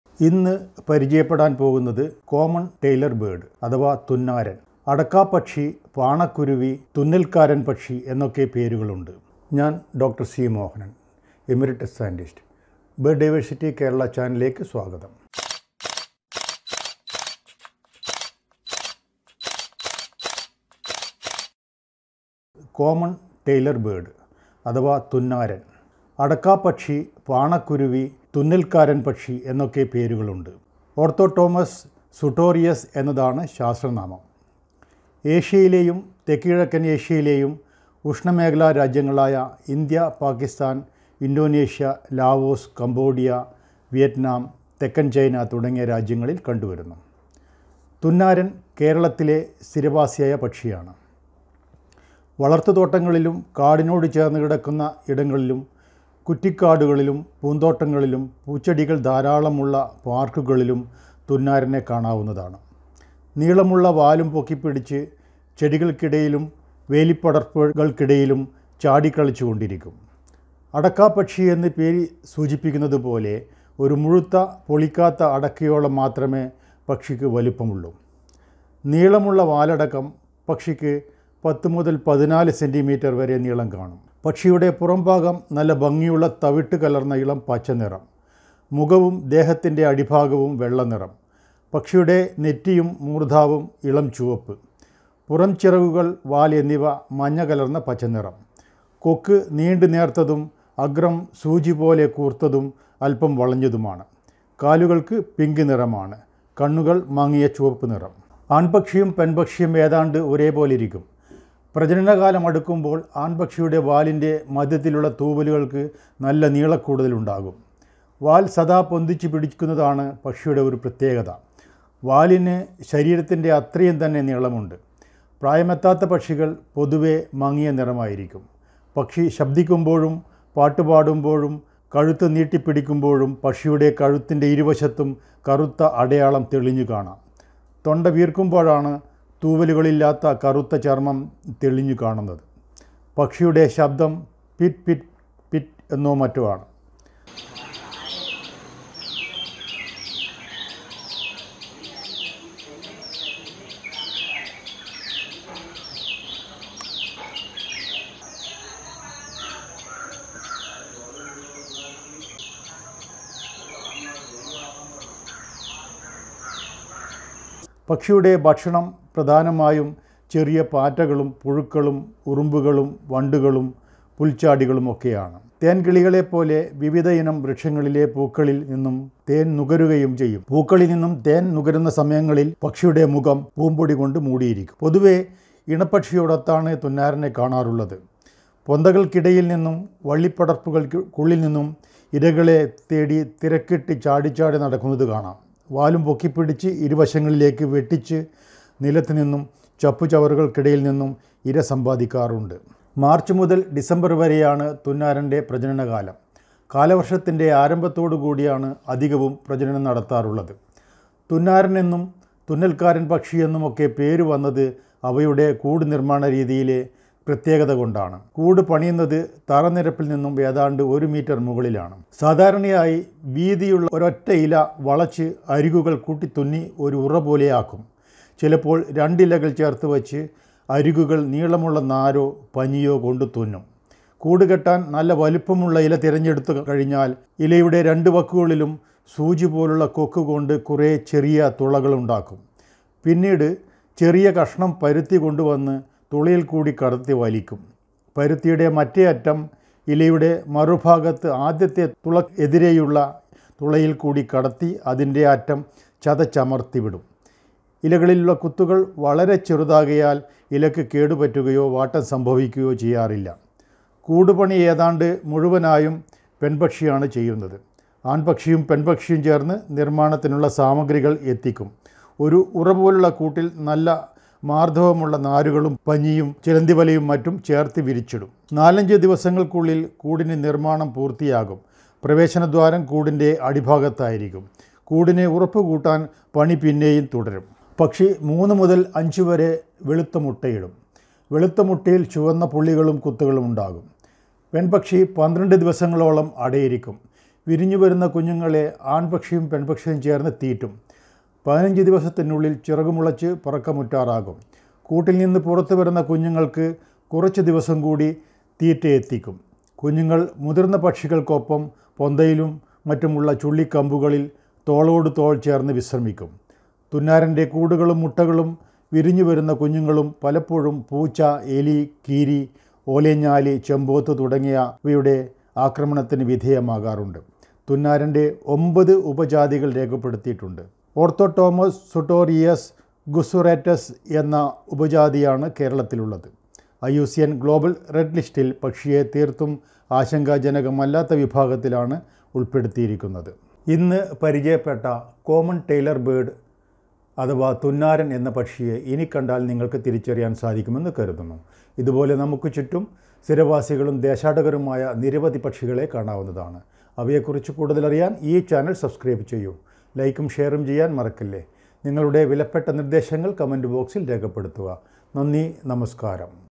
Common Tailorbird | Orthotomus sutorius | Thunnaran | തുന്നാരൻ | അടയ്ക്കാപ്പക്ഷി | തുന്നൽക്കാരൻപക്ഷി
Today we are going to familiarize the bird, Common Tailor bird.  In ‘Malayalam’ vernacular,  the bird is called as “ Thunnaran” or ‘Adakkapakshi”.  Also called as “Panakkuruvi”  or ‘Thunnalkaran Pakshi”
Scientific name of the bird is  Orthotomus sutorius .
Common Tailor bird is distributed across tropical Asian and southeast Asian countries including India, Pakistan, China, Indonesia, Laos, Cambodia, Vietnam.  Common tailor bird is resident to Kerala and occurs in open farm land, scrub, plantations, forest edges and  gardens and parks.
Common Tailor Bird is a small bird range in size from 10 to 14 cm. The bird  is a brightly coloured with bright green upperparts and creamy underparts.  The crown is rufous and the upperparts are predominantly olive green.  They have short rounded wings, a long tail, strong pink colored legs and a sharp bill with curved tip to the upper mandible. The bird has  a long upright tail that is often moved around. The underside is creamy white. The sexes are identical, except that the male has long central tail feathers  in the breeding season.
 Young birds are duller.  When calling, the dark patches on the sides of the neck become visible.  These are due to the dark pigmented and bare skin that are present in both sexes and sometimes give the appearance of a dark gorget.
Song, a loud, repeated, rather sudden outburst, “chubit, chubit, chubit, chubit”, or as “pitchik, pitchik, pitchik, pitchik” and so on.  Frequently heard calls include loud, rapidly repeated “pit-pit-pit” in agitation, and repeated “cheep, cheep, cheep, cheep” contact during foraging.
Food almost entirely tiny invertebrates, chiefly insects, including small beetles, bugs, ants and jassid flies, as well as small butterflies and moths. Also feeds on nectar, especially of Salmalia, Bombax, Erythrina and Butea, but visits a number of other species of flowering trees and shrubs.  When feeding on flowers, the bird’s face may become discoloured by pollen. The birds are seen usually in pair , feeding within cover; works its way up through tangled vegetation, vines etc. Often forages on ground under cover of shrubs , hopping with tail held cocked and flicked from side to side. Flies weakly, with tail twitching, moving only short distances between bushes.
The breeding season is March to December peaking from June to August in Kerala, coinciding with the rainy season.
The name of the bird is derived from their nest construction habit.  Generally found within 1 m above the ground level.  Usually  the nest is made from a single large leaf, the margins of which are rivetted together. Sometimes the fibres from one rivet are extended into an adjoining puncture and appearing more like sewing.   The stitch is made by piercing two leaves and drawing fibre through them. The fibres fluff out on the outside and in effect they are more like rivets.  The punctures made on the edge of the leaves are minute and do not cause browning of the leaves, further aiding camouflage.

 The nest is a deep cup, lined with soft materials, fibres, cotton  and placed in thick foliage and the leaves holding the nest have the upper surfaces outwards making it difficult to spot.  The nest is constructed by the female bird using materials brought by the male bird.  Within 4 to 5 days the nest construction will be completed.   Also do the repair frequently.  Clutch 3–5 eggs, usually three, white eggs with reddish brown spots and patches. Incubation is by both sexes.  Incubation period is 12 days; nestlings fed by both sexes. Young birds fledge within 15 days. Mortality of eggs and chicks is high due to predation by rodents, cats, crow-pheasants, lizards and other predators.
The birds roost alone during the non-breeding season but may roost side-by-side during the breeding season, sometimes with the newly fledged juvenile sandwiched between the adults. The roost sites chosen are thin twigs on trees with cover above them and were often close to human habitation and lights.
Nine subspecies  have been recognized and  Orthotomus sutorius guzuratus occurs in peninsular India including Kerala.

In IUCN Global red list, the bird is included under “Least Concern’.